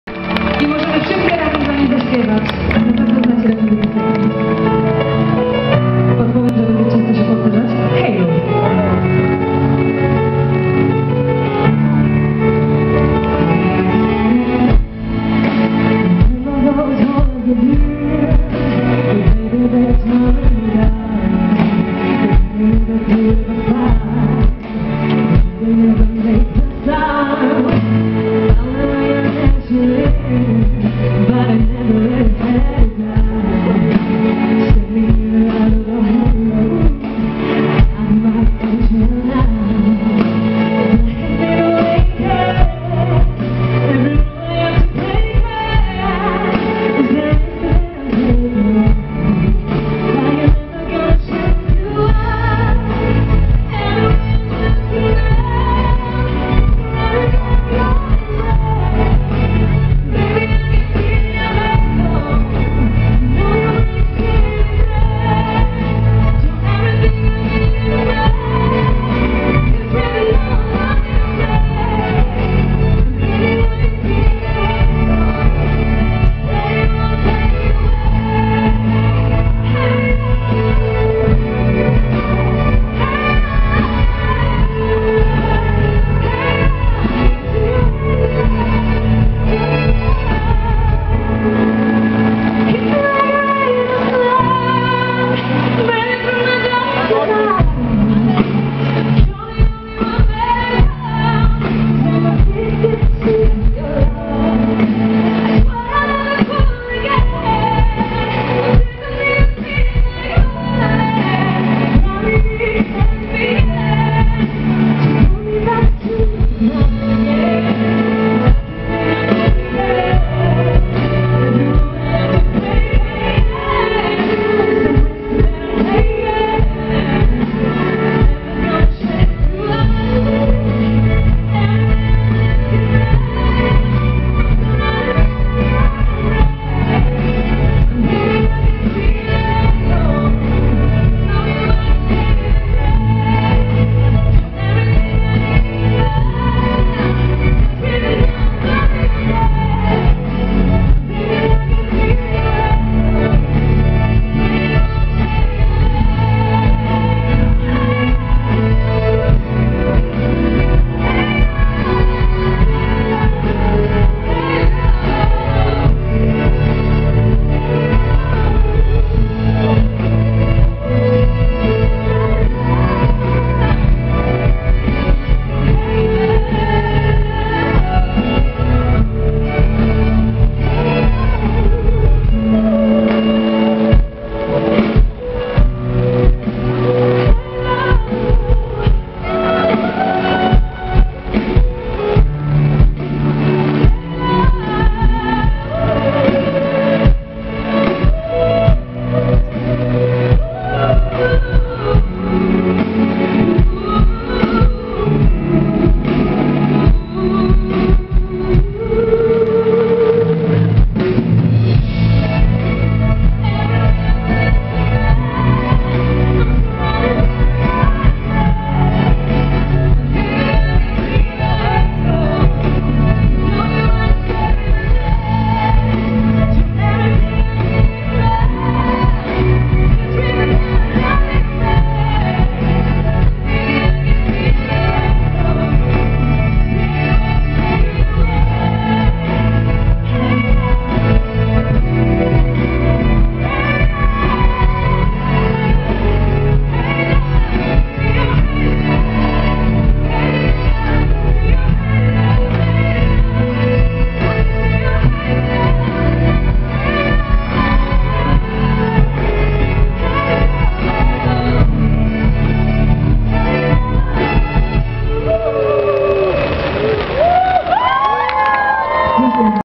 Borzęckie Święto Grzyba 2016
Koncert: ROCK SYMPHONY - Koncert Polskiej Orkiestry Muzyki Filmowej (SOLIŚCI: Juan Carlos Cano, Michał Gasz, Zofia Nowakowska, Justyna Janik, Agnieszka Twardowska, Judyta Wenda, Anna Hnatowicz, Marcin Molendowski, Jakub Jurzyk).